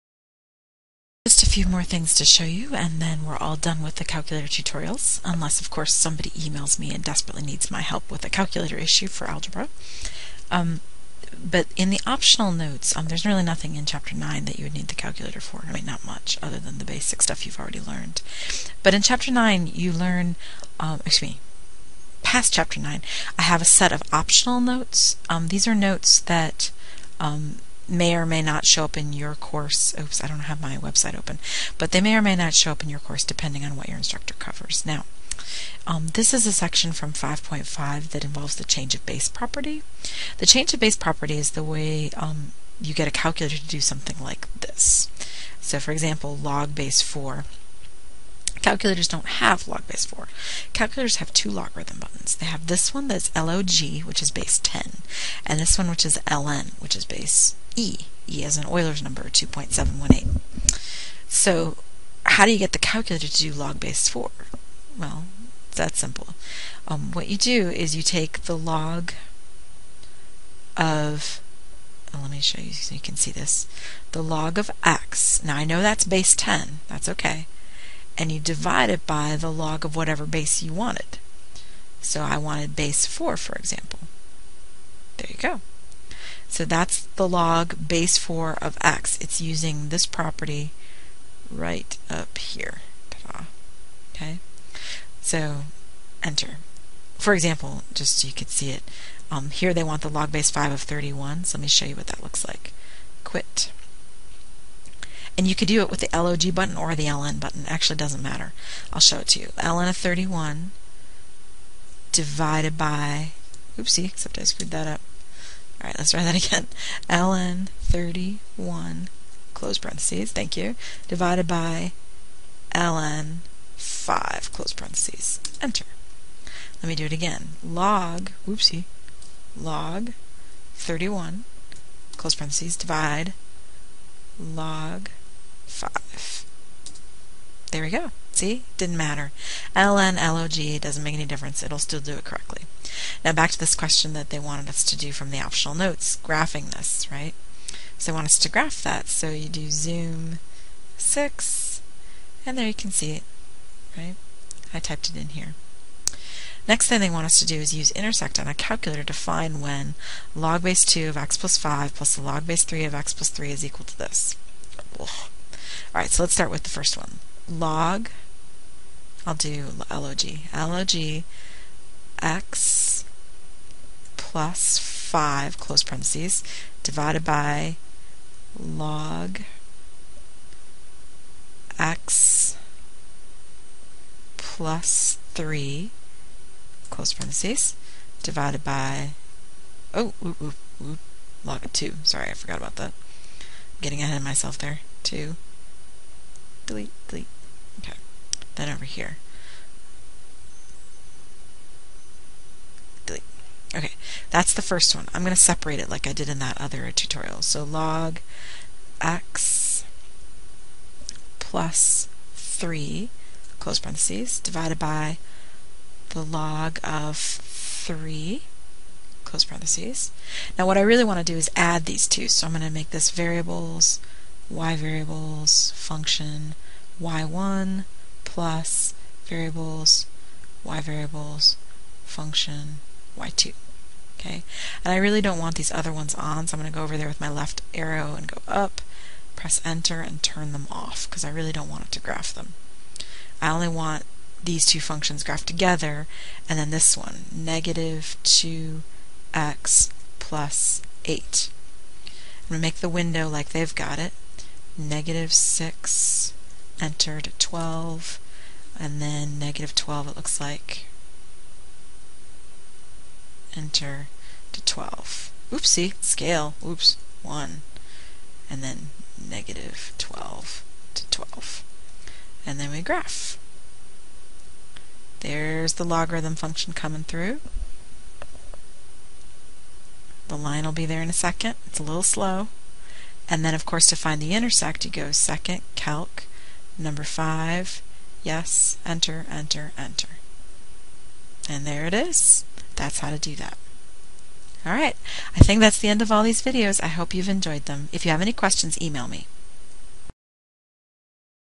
Ch 5 - Part 1 of 1 - Math 131 TI-83/84
Using a Ti-83/84 calculator in Math 131: Intermediate Algebra. This tutorial covers:

Using the Change of Base Property to find any logarithm
Using VARS to graph a complicated logarithm function
Using CALC to find the intersection of a logarithm and a line

Primarily meant for Math 131 students at Jackson Community College, MI. Textbook is Intermediate Algebra: Functions and Authentic Applications by Jay Lehmann, 4th edition.